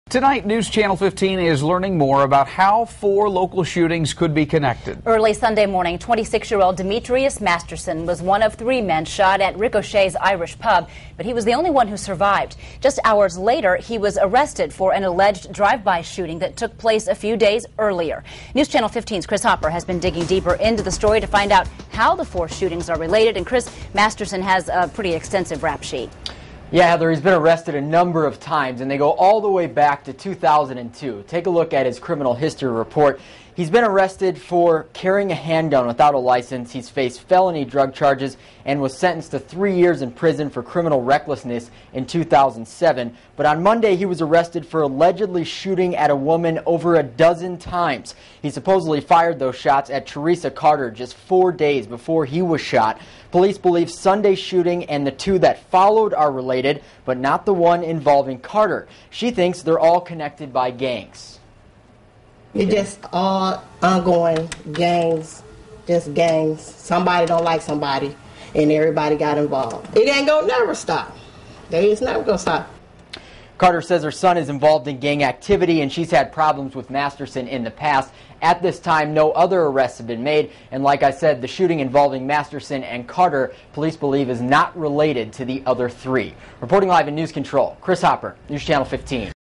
Shootings Connected? @ 6pm
Four recent shootings in Fort Wayne could have a strong connection.